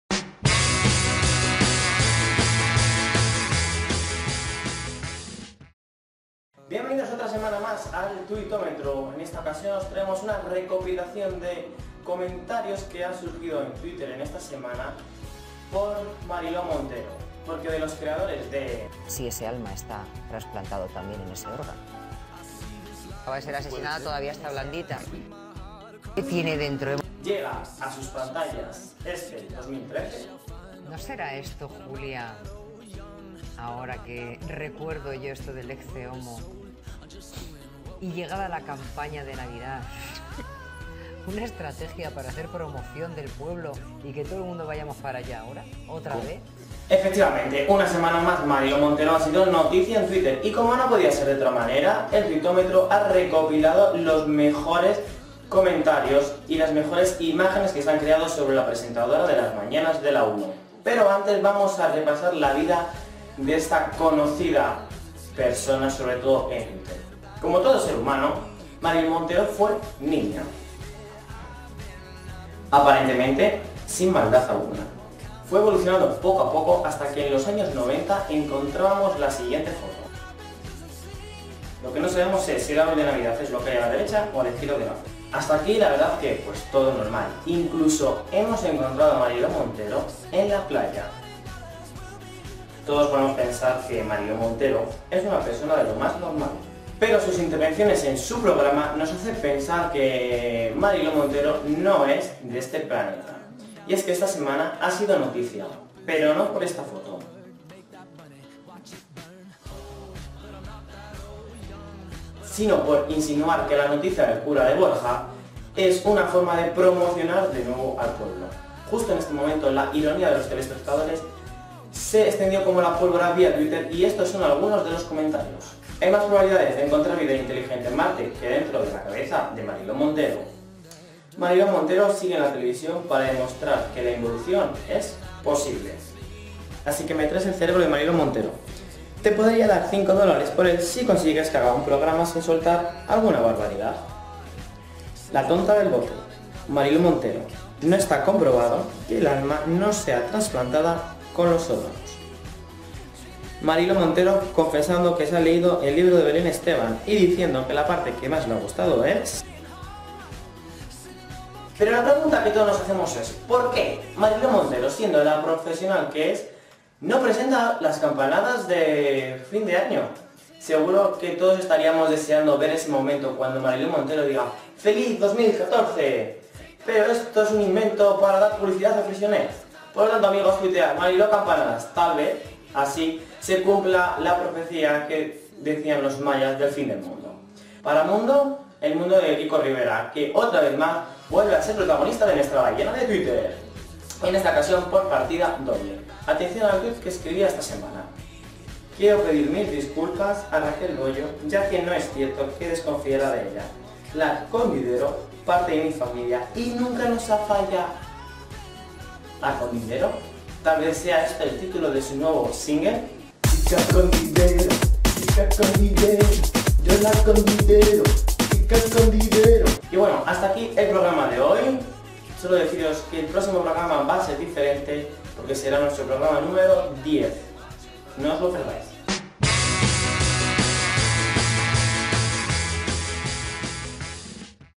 El Tuitómetro | Programa 9
Copyright ElTuitómetro es una sección de ElTelevisero.com presentada por Víctor Nieto. Noveno programa en el que repasamos los mejores tweets que se escribieron la semana pasada, tras la nueva "metedura de pata" de la presentadora de 'La mañana de La 1' Mariló Montero, al insinuar que la noticia del cura de Borja, podría ser una nueva forma de reclamar turistas para el pueblo que se hizo famoso por el Ecce Homo.